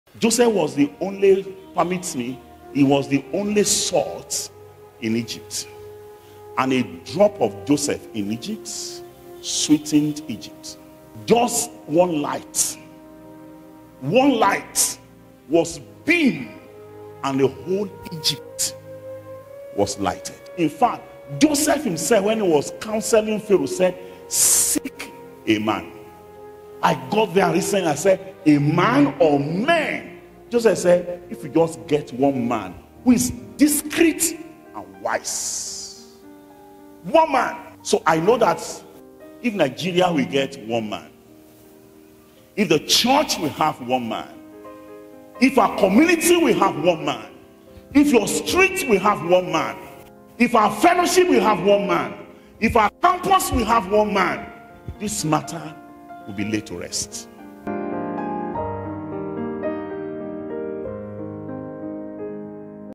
Joseph - A Light!
Joseph was the ONLY 'light' in Egypt, yet the entire land was lighted! How many lights from our phone can lighten a 1000-seater auditorium? Even if everyone has one, the hall will still not be bright enough.
Though you may be ALONE in that organization, your burning and shining light could make a difference if the size of your light and its luminance is stronger than the darkness present there, otherwise you will need assistance!
Why not ask God to increase the size and the brightness of your light?
Shalom!